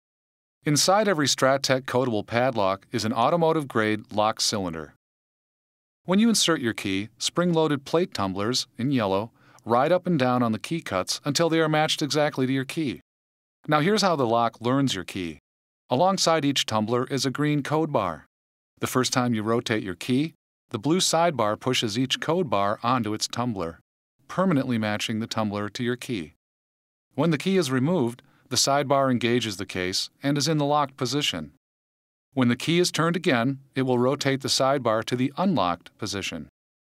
How the BOLT padlock works
This is a brief video demonstration on how the BOLT Padlock works.